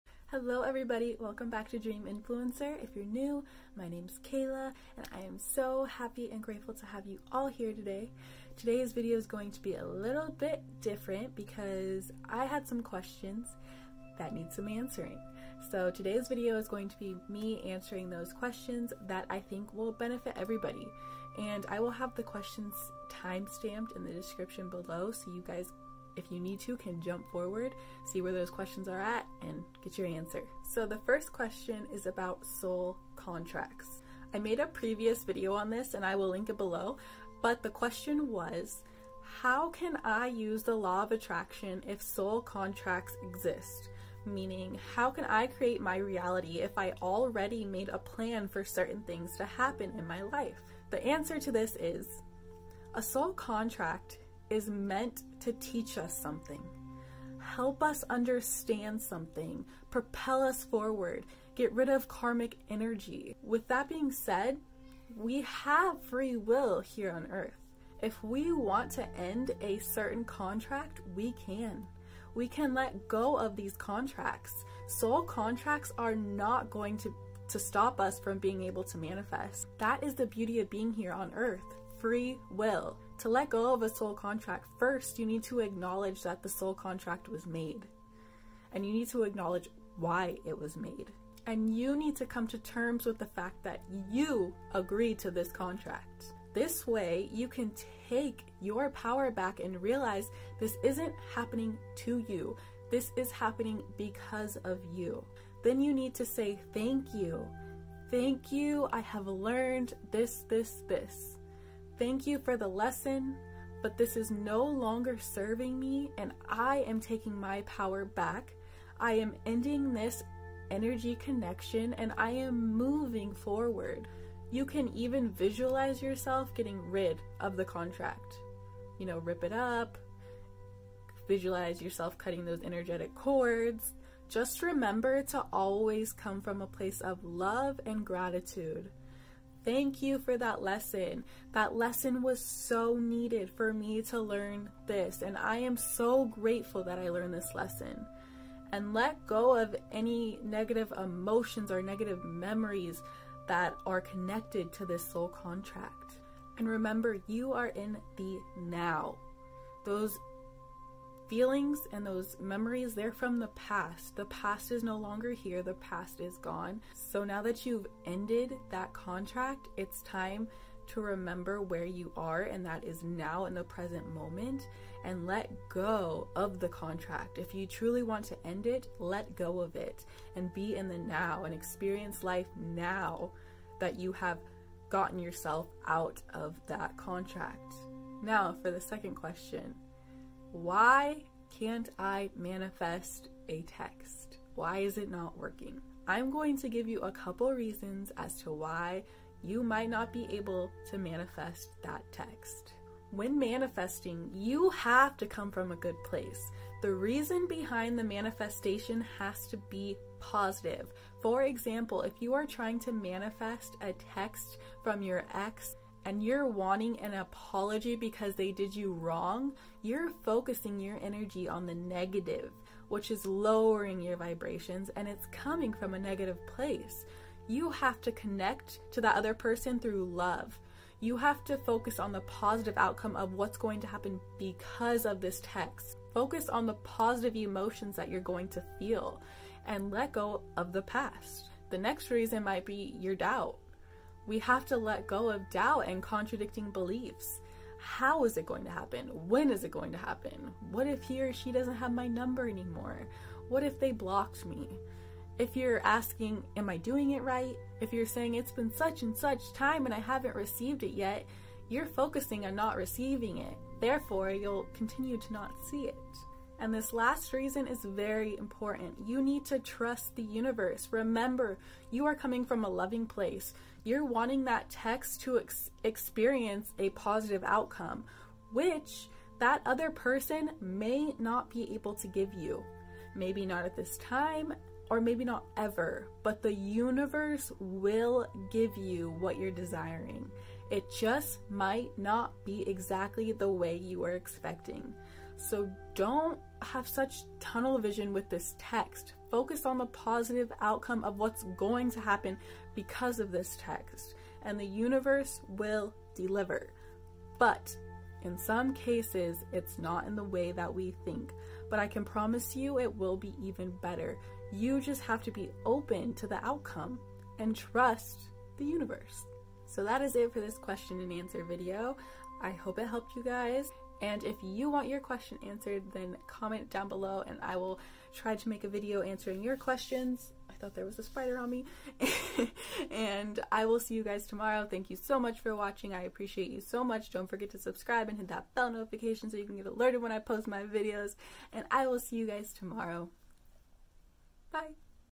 Q&A: WHY CAN'T I MANIFEST A TEXT I SOUL CONTRACTS & THE LAW OF ATTRACTION
Hey loves!

In today's Q&A I will answer the questions "Why can't I manifest a text?" and "How can I use the Law of Attraction if soul contracts exist?"

Soul Contract Answer: 0:40
Manifest Text Answer: 3:09